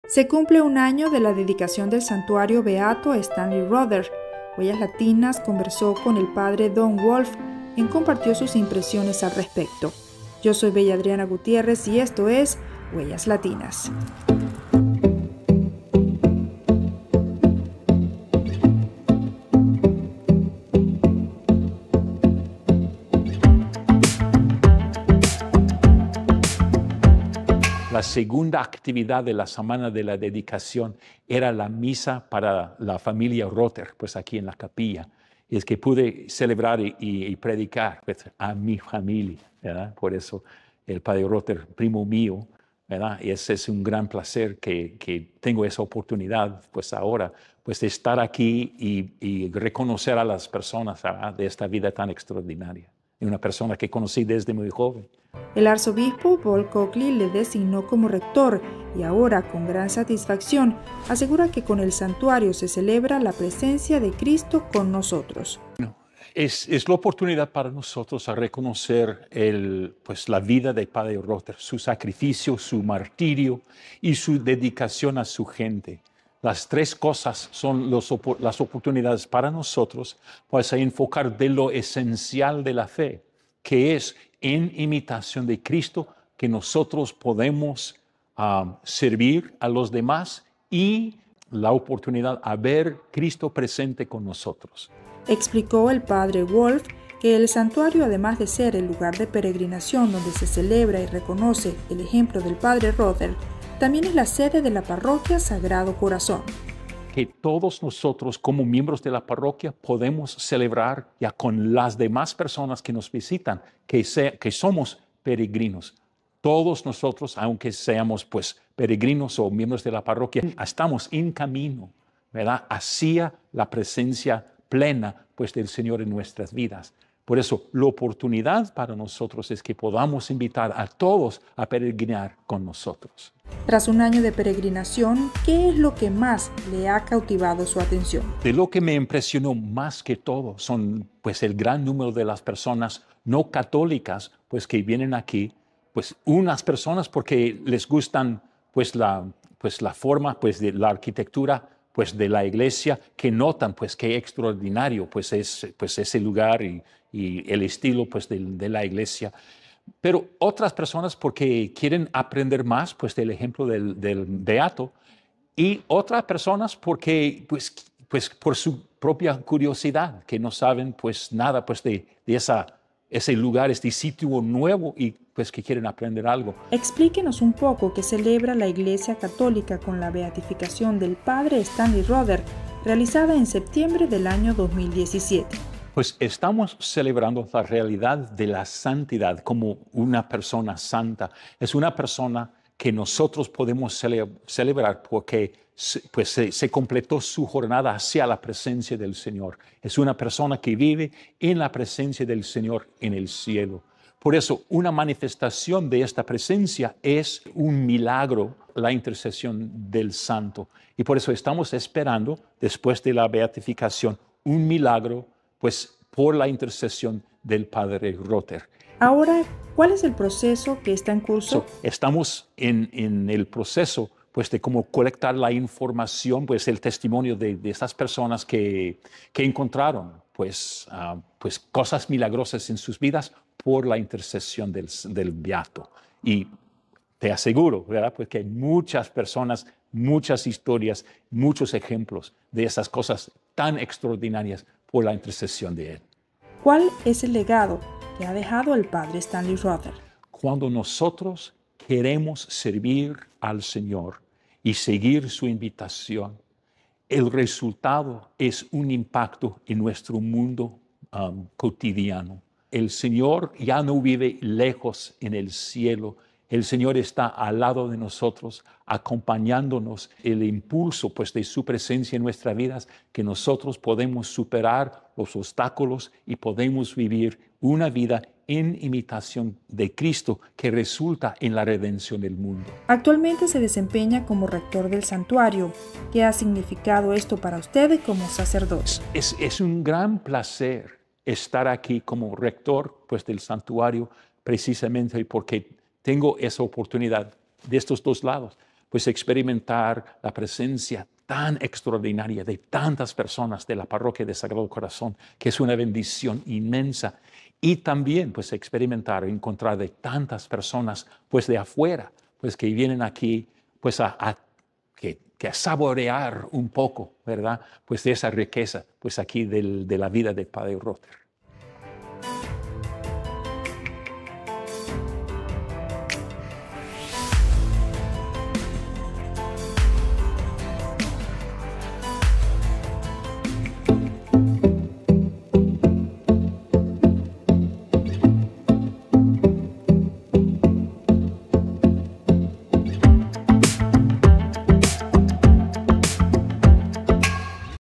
Beato Stanley Rother Shrine. Primer aniversario
Se cumple un año de la Dedicación del Santuario Beato Stanley Rother. Conversamos con el Padre Don Wolf al respecto.